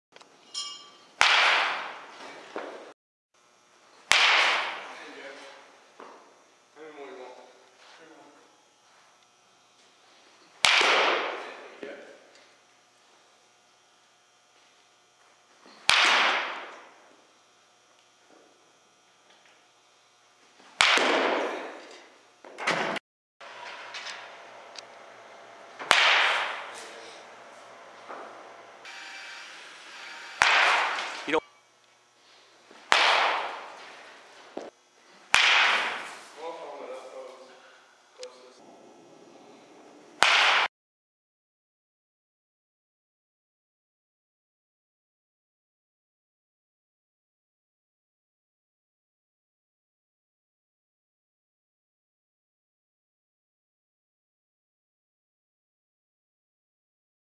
Nick Wise NEW Hitting In Cages 3/9/2014
Alexander High School
Douglasville, GA
OF,1B,LHP